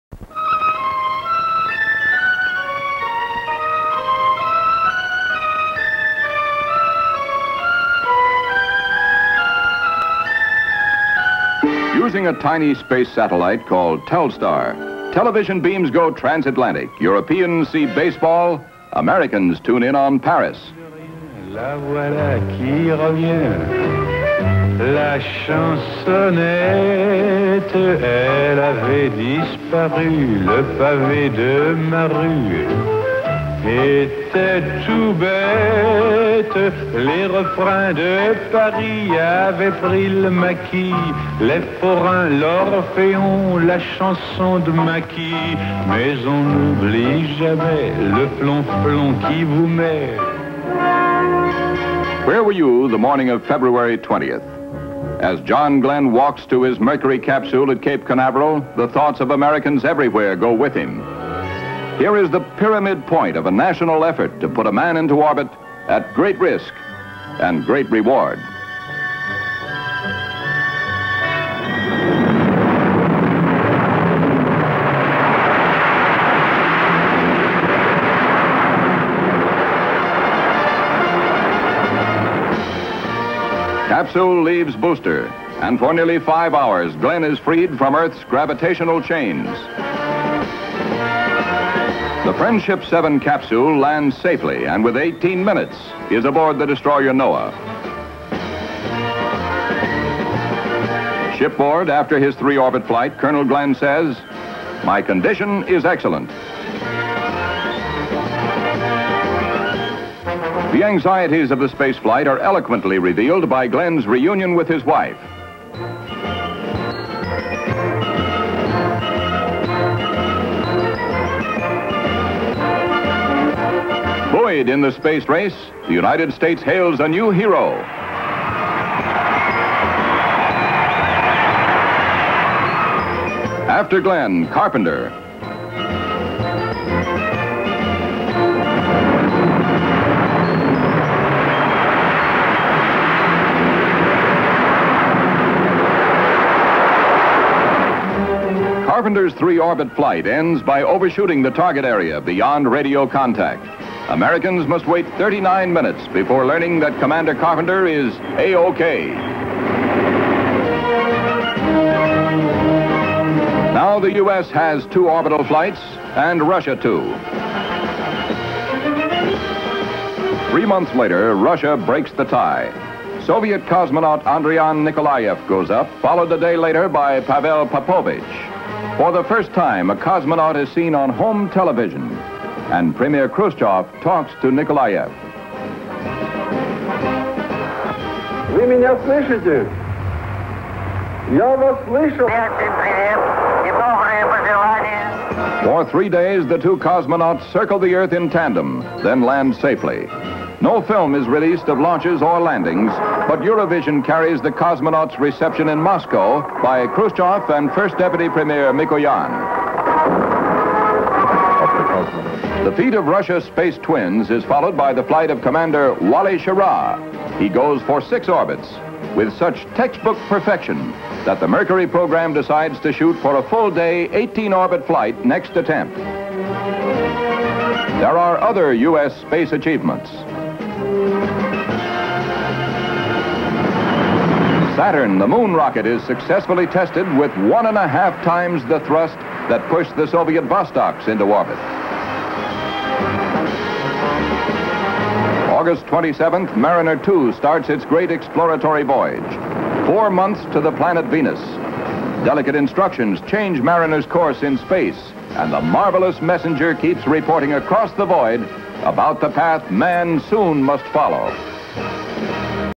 1962 Space Race (stock footage / archival footage).
Rocket into space. Animation of Telstar. Yves Montand in first transcontinental TV broadcast. Telstar ground receiving station seen. John Glenn walks to capsule. Friendship 7 takes off (NICE), view of Earth, Glenn in capsule, splashdown, off plane, waves and smiles. Ticker tape parade. Scott Carpenter into space on rocket, view inside capsule, Carpenter rescued from water. People in Red Square in Moscow. People gather around car radio. TV transmission of cosmonaut Andriyan Nikolayev in space. Khrushchev on phone speaks to Nikolayev. Khrushchev hugs cosmonauts in Red Square (off TV). Wally Schirra in orbit. Animation of orbits around Earth (very 1960s). Saturn 5 takes off (NICE). Mariner 2 takes off at night (to Venus). Animation of satellite view of Earth.